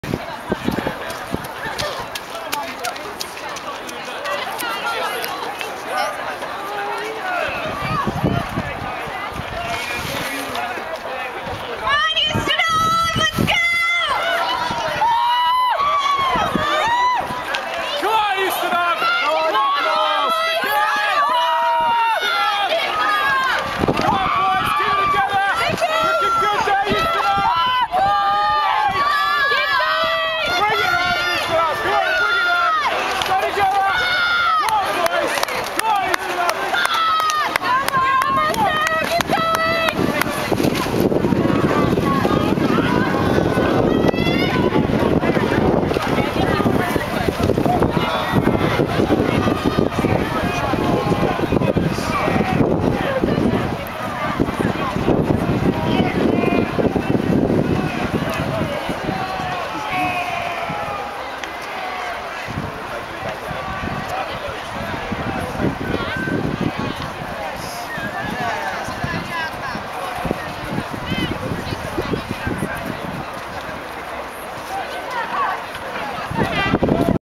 10. Ustinov Men - Howie - Race 2 (Day 2).AVI
Durham University Ustinov College Men compete (and win) their second rowing race of the November, 2008, Novice Cup